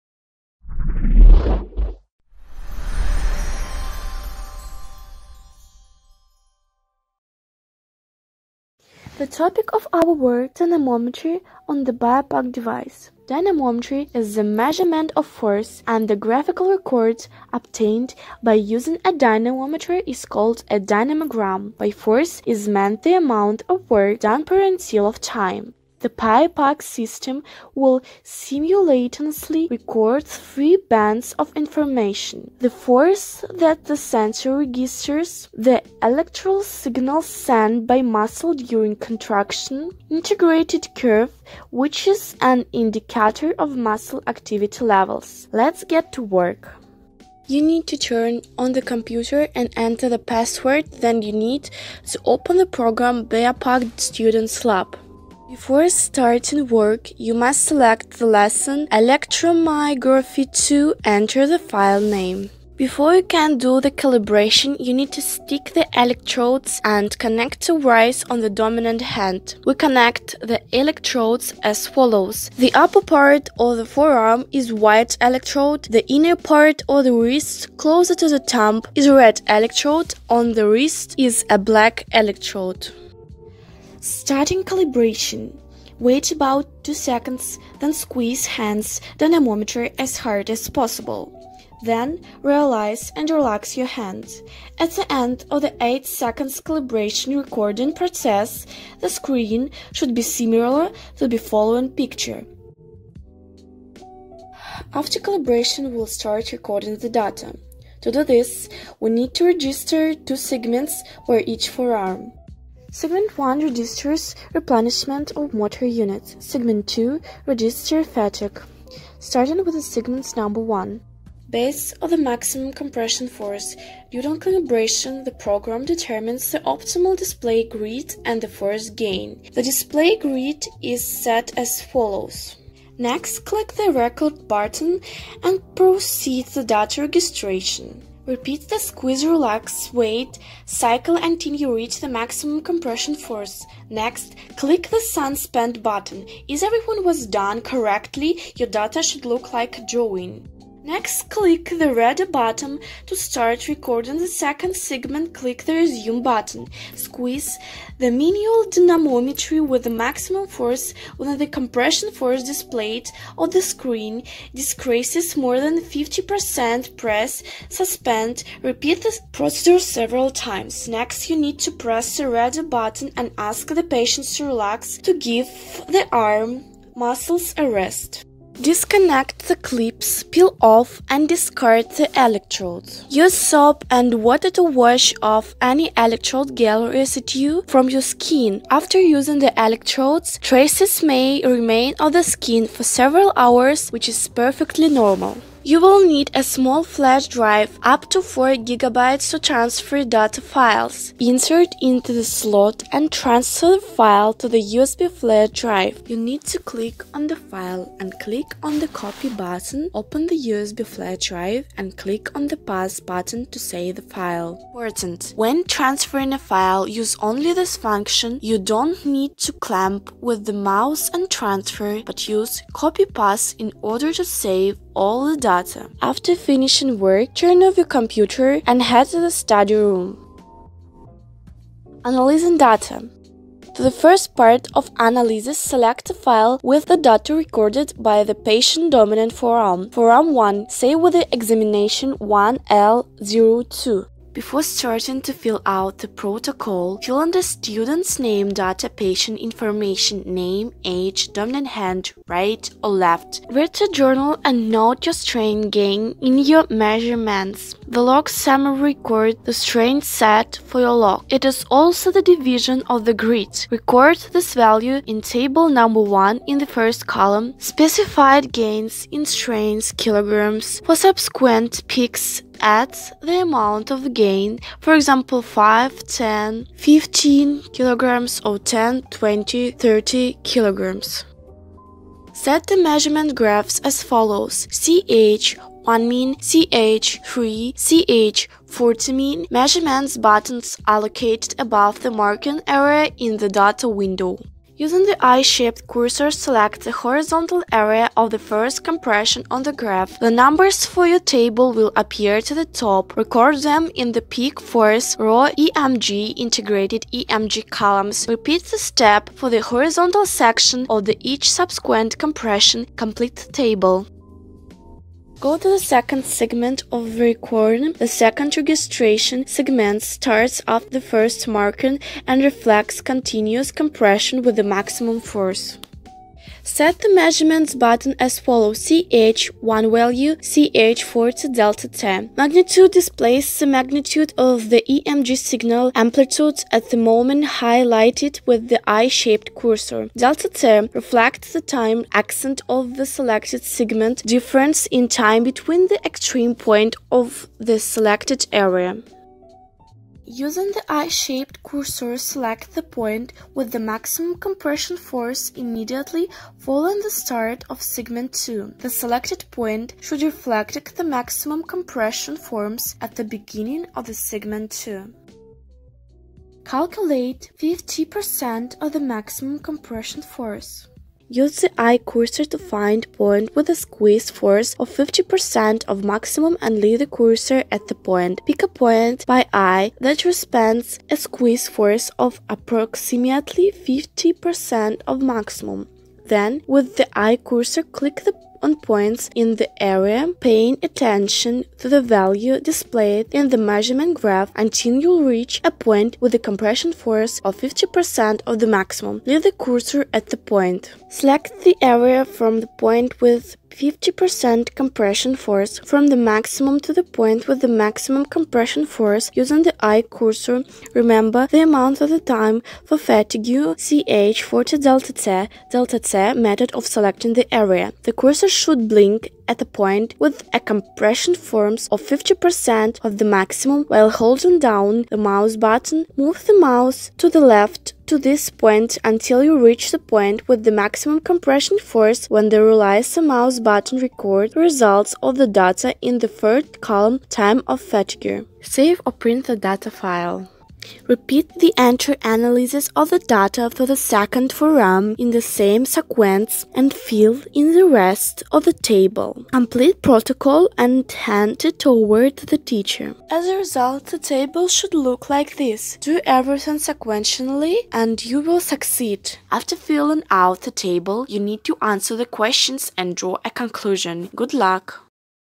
Dynamometry
This is a video for laboratory work with the BIOPAC Student Lab program for students studying in English in the specialty "Dentistry" and "General Medicine"